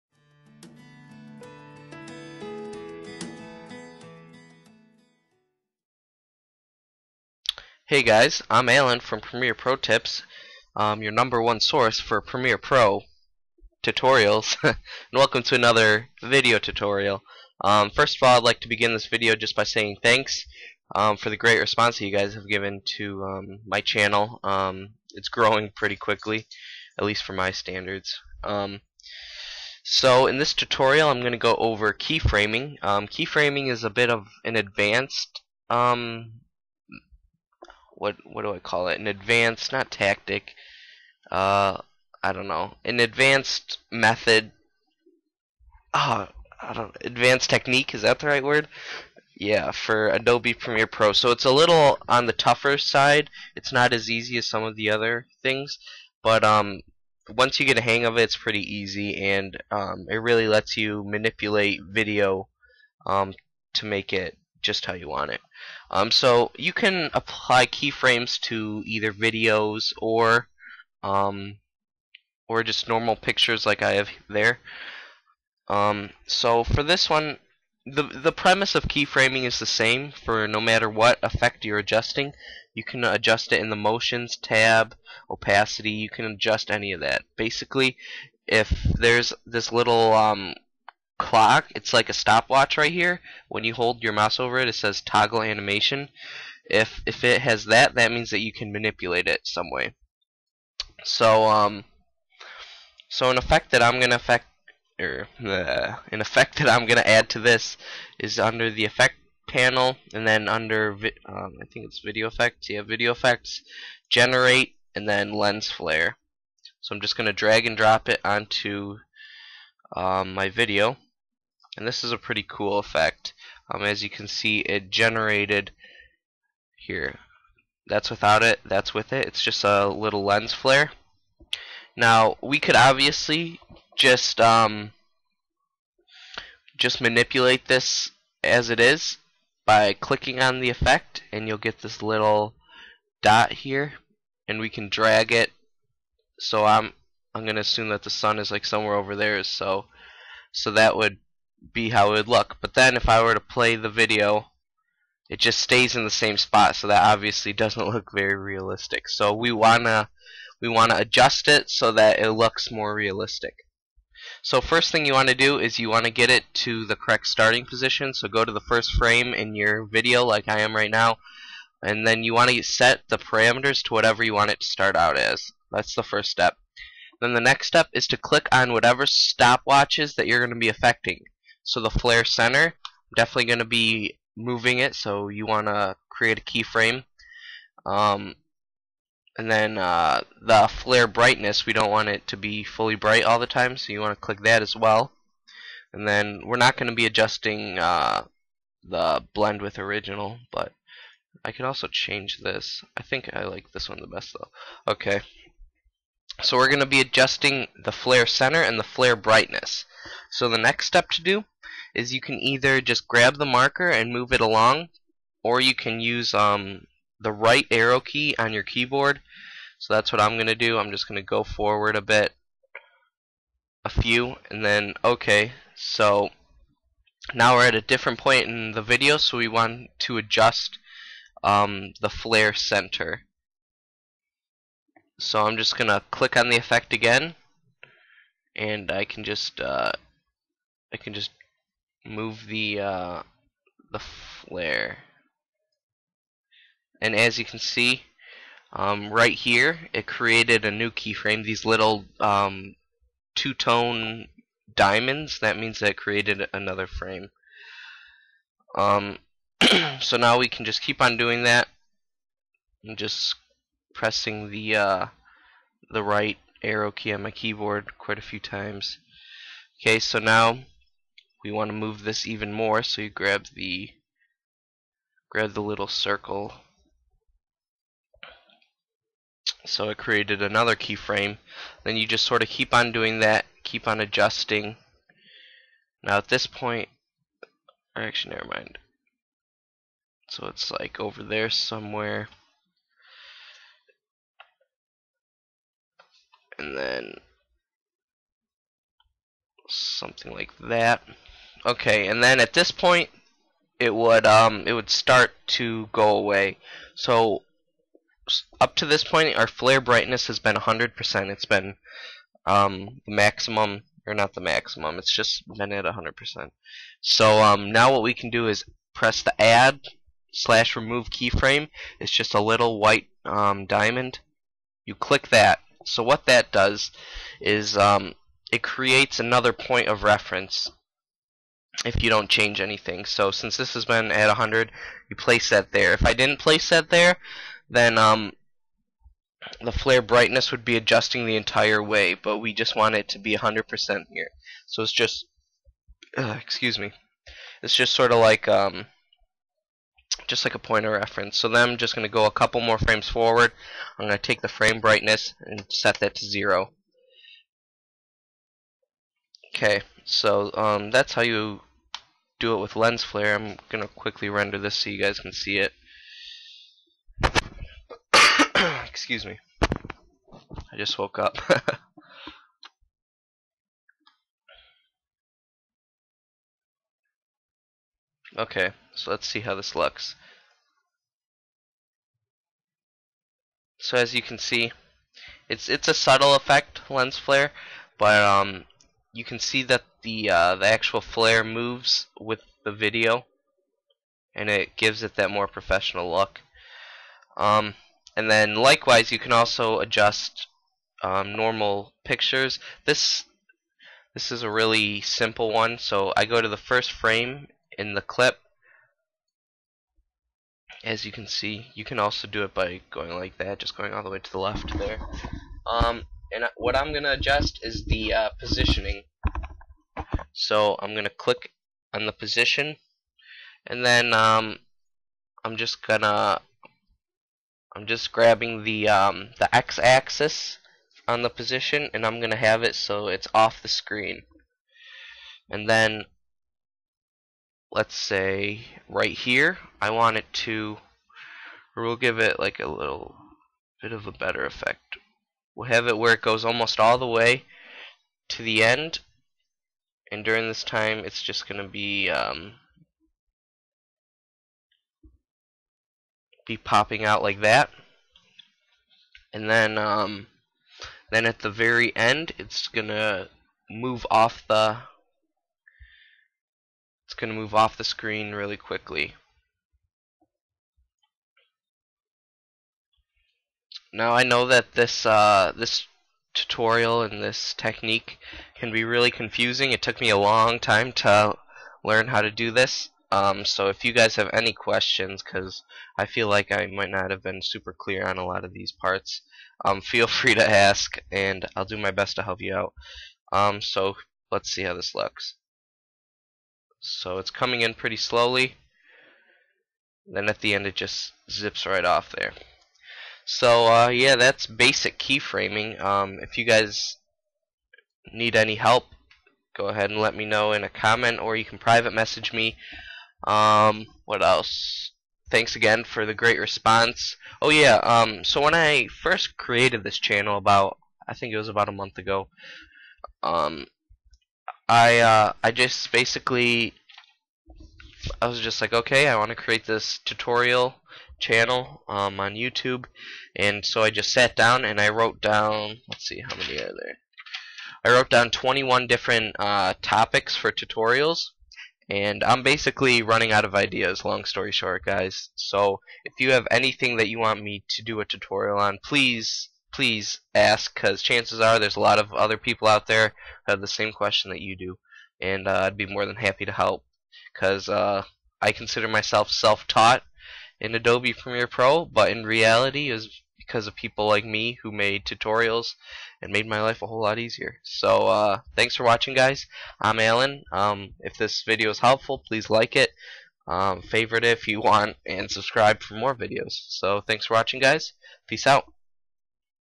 Keyframing Tutorial - Adobe Premiere Pro
In this video, I explain how to use the advanced technique of keyframing in Adobe Premiere Pro. If you have any questions, leave a comment or send me a private message. Thanks for the support guys!